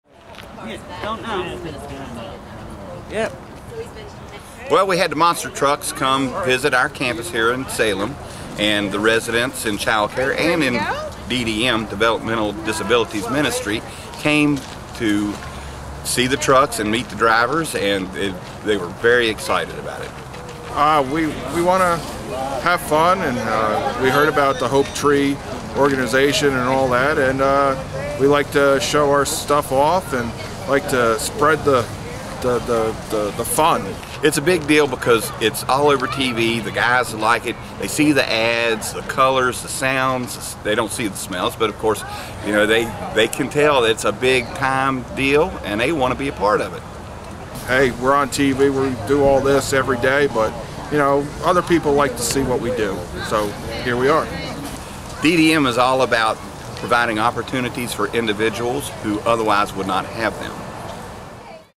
Viper Monster Truck visits HopeTree
The Viper Monster Truck visited HopeTree Family services recently, and made a HUGE impression! Special thanks to Rich Blackburne and Team Viper for coming out!